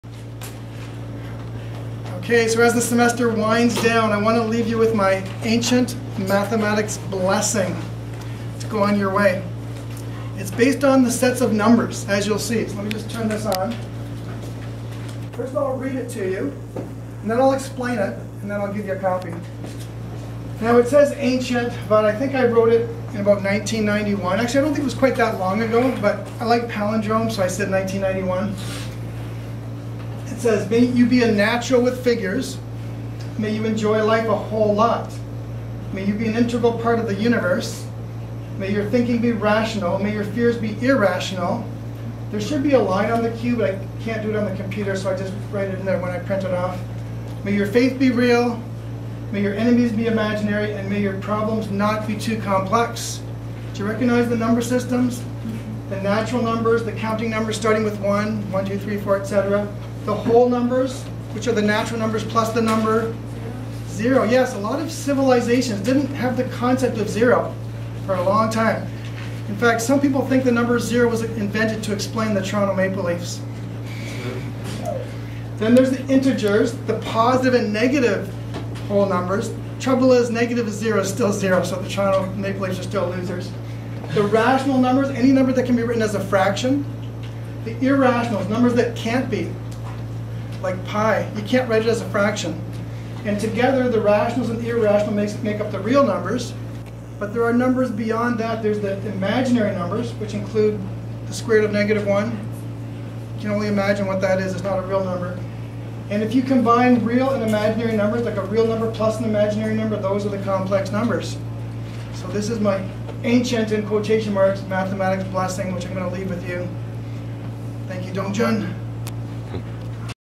Ancient Mathematics Blessing (circa 1991)
Mr. John Hush reveals the Ancient Mathematics Blessing that he bestows on each of his students at the end of each semester. It's not so ancient, having been written by him in the early nineties, officially 1991, a palindromic year, but probably a few years after that. It is based on the different sets of numbers that mathematicians use to categorize all numbers.